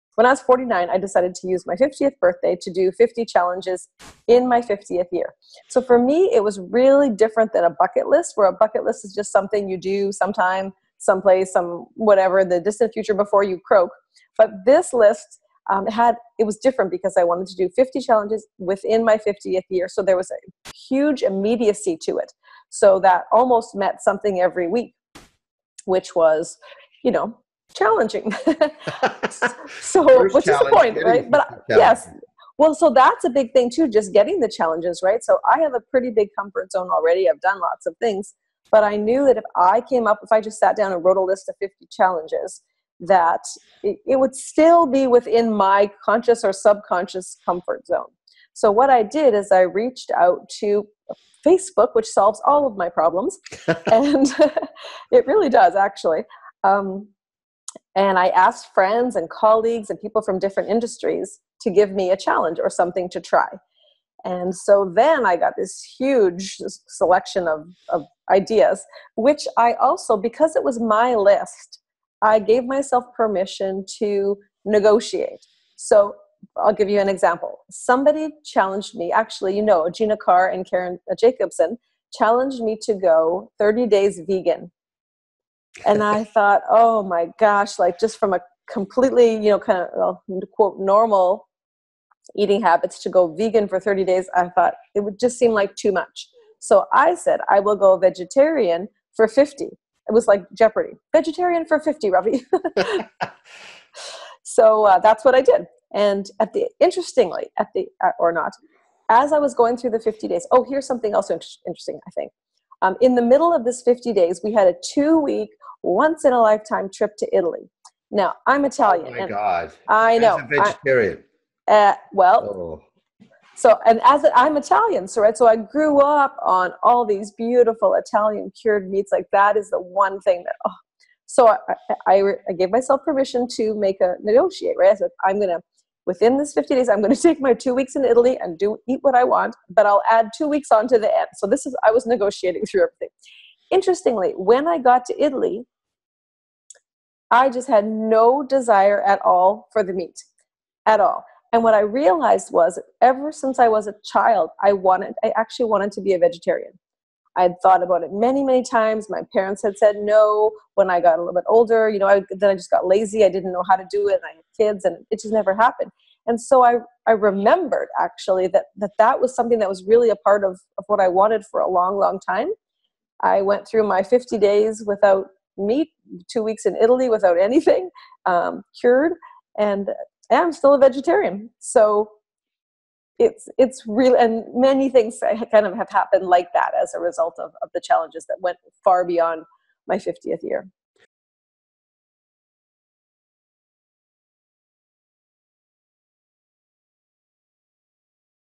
StephanieStaples meh2YEAH Pro File Highlights 180306
When life gets busy, we can get locked into routine and it's often hard to even think what we could do to break out of it.  Stephanie Staples decided to take on 50 challenges for the year of her 50th birthday, and some of those transformed her life permanently.  Discover how Stephanie has learned to live her life unlimited - and how you can push your boundaries to break out of everything that's familiar - and sometimes "meh" - as well.